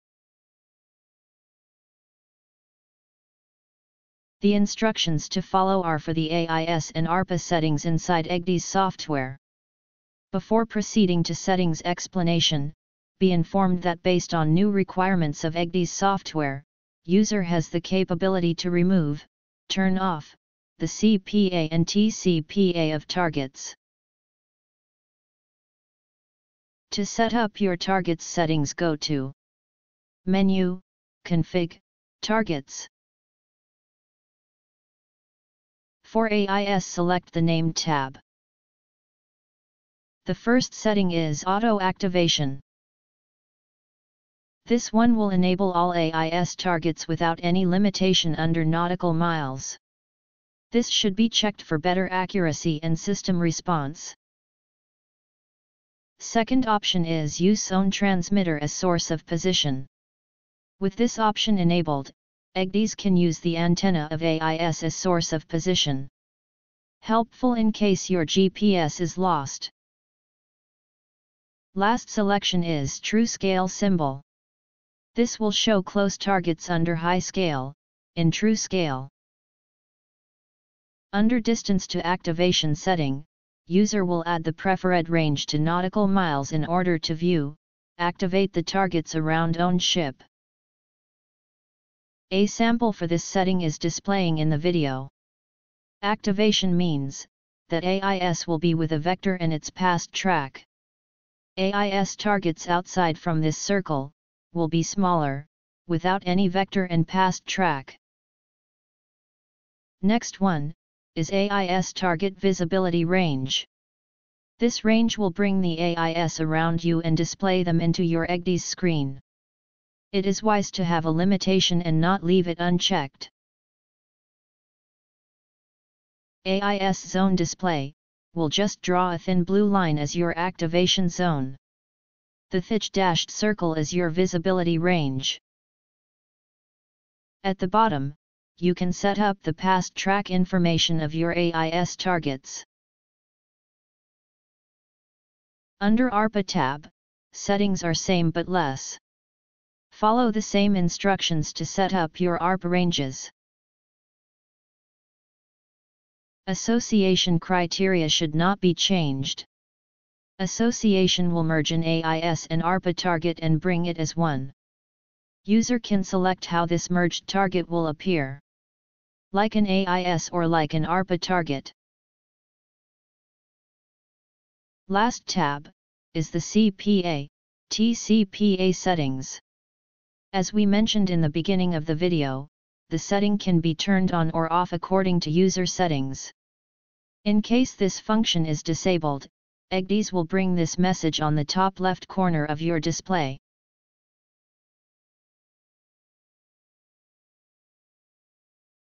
AIS ARPA targets
New features of AIS and ARPA targets in version 7.1